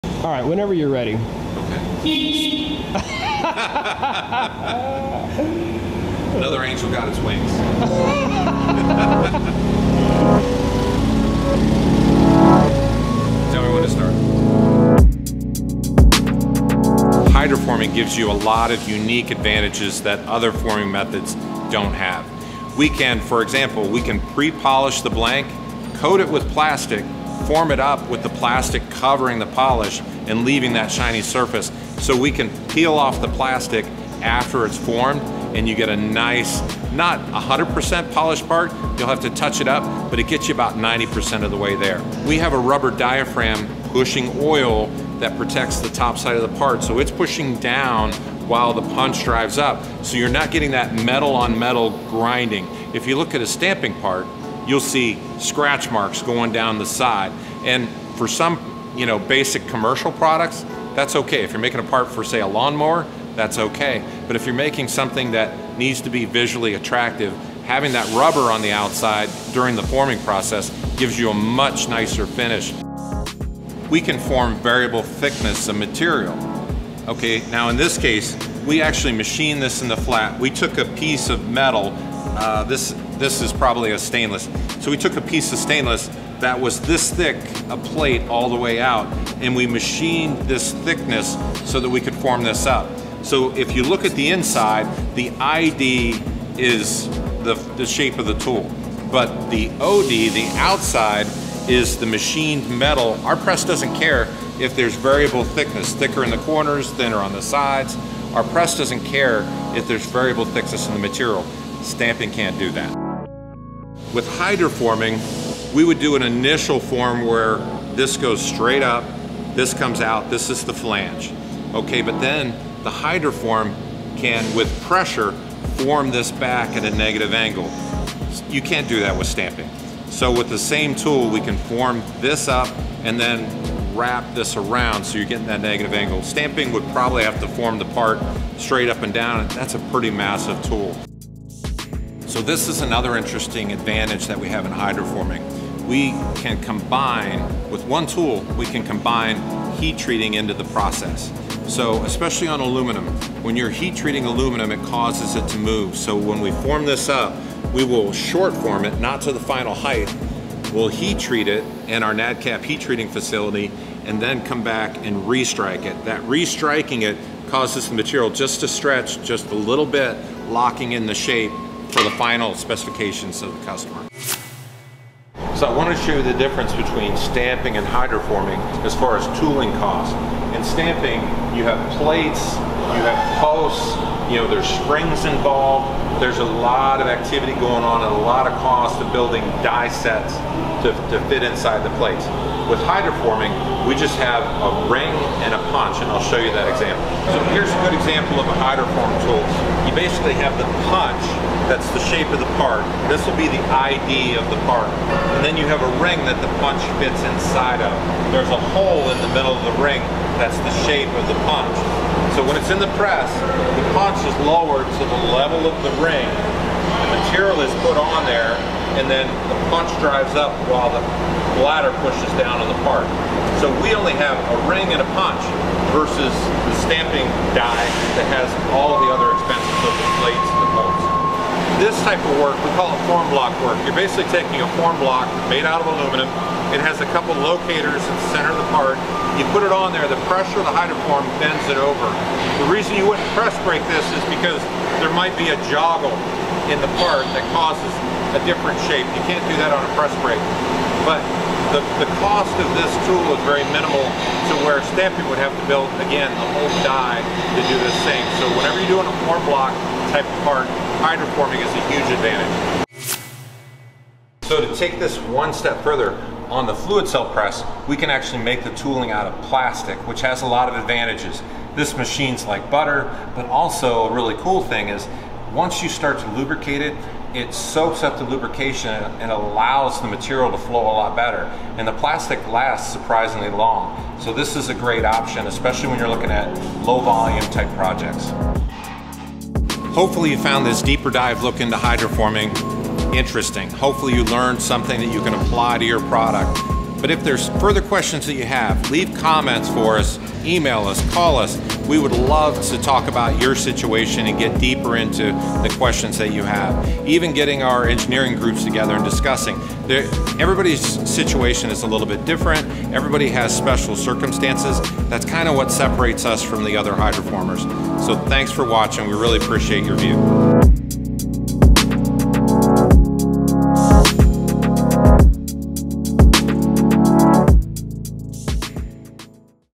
Top 6 Hydroforming Advantages | Jones Metal
President & CEO of Jones Metal, Dan Erb:

"Hydroforming gives you a lot of unique advantages that other forming methods don’t have.

We can for example, we can pre-polish the blank, coat it with plastic, form it up with the plastic covering the polish and leaving that shiny surface. So we can peel off the plastic after it’s formed and you get a nice, not 100% polished part, you’ll have to touch it up, but It gets you about 90% there.

We have a rubber diaphragm pushing oil that protects the top side of the part.
So it’s pushing down while the punch drives up, so you’re not getting that metal-on-metal grinding. If you look at a stamping part, you’ll see scratch marks going down the side. For some basic commercial products, that’s okay. If you’re making a part for, say a lawn mower, that’s okay, but if you’re making something that has to be visually attractive, having that rubber on the outside during the forming process gives you a much nicer finish.

We can form variable thickness of material.

We actually machined this in the flat. We took a piece of stainless that was this thick, a plate all the way out, and we machined this thickness so that we could form this up.
So if you look at the inside, the ID is the shape of the tool, but the OD, the outside, is the machined metal. Our press doesn’t care if there is variable thickness, thicker in the corners, thinner on the sides… our press doesn’t care if there is variable thickness in the material. Stamping can’t do that.

Negative Angles
With hydroforming, we would do an initial form where this goes straight up, this comes out, this is the flange, but then the hydroform can, with pressure, form this back at a negative angle. You can’t do that with stamping.

So with the same tool, we can form this up, and then wrap this around, so you get this negative angle. Stamping would probably have to form the part straight up and down and that’s a pretty massive tool.

This is another interesting advantage that we have in hydroforming… we can combine (with one tool) we can combine heat treating into the process. Especially on aluminum, when you’re heat treating aluminum it causes it to move, so when we form this up, we will short form it, not to the final height. We’ll heat treat it in our NADCAP heat treating facility, and then come back and restrike it. That restriking it causes the material to stretch just a little bit, locking in the shape for the final specifications of the customer.

I want to show you the difference between stamping and hydroforming as far as tooling cost.  In stamping you have plates, posts, and springs.  There's a lot of activity going on and a lot of cost in building die sets to fit inside the plates.  With hydroforming we have a ring and a punch.  Here's a good example of hydroform tools.  You have the punch that's the shape of the part.  This will be the ID of the part.  Then you have a ring the punch fits inside of. There’s a hole in the middle of the ring that’s the shape of the punch.

When it's in a press the punch is lowered to the level of the ring and material is placed on the ring.  The punch drives up while the bladder pushes down on the part.  We only have a ring and a punch vs the stamping die that has all the other expense of the plates and the posts.

This type of work we call it form block work. You’re basically taking a form block made out of aluminum. It has a couple of locators in the center of the part. The pressure of the hydroform bends it over.

You wouldn’t press break this is because there might be a joggle in the part that causes a different shape you can’t do that on a press break, but
The cost of the tool is very minimal to where stamping would have to build the whole die to do this. SO whenever you’re doing a form block, hydroforming is a huge advantage.

So to take this one step further… on the fluid cell press, we can actually make the tooling out of plastic which has a lot of advantages. This machine’s like butter, but one really cool thing is once you start to lubricate it, it soaks up the lubrication and allows the material to flow a lot better and the plastic lasts surprisingly long so this is a great option, especially when you’re looking at low volume type projects.

Hopefully you found this deeper dive into hydroforming interesting, hopefully you learned something you can apply to your product. But if there are further questions that you have, leave comments for us, email us, call us, we would love to talk about your situation and get deeper into the questions you have.

Even getting our engineering groups together and discussing. Everybody’s situation is a little different. Everyone has special circumstances. That’s kind of what separates us from other hydroformers.